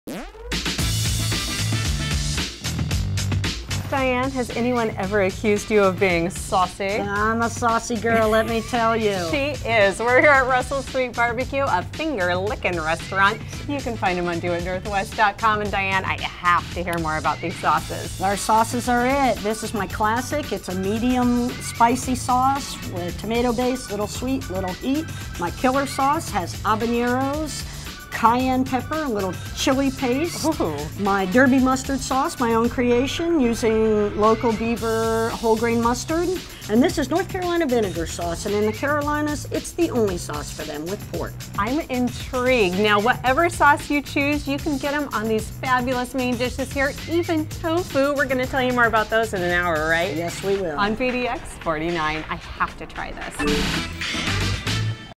Dining Out in the Northwest: Russell Street BBQ - Portland, Oregon (5)
Dining Out in the Northwest: Russell Street BBQ - Portland, Oregon. To discover more delicious local restaurants in Oregon and Washington,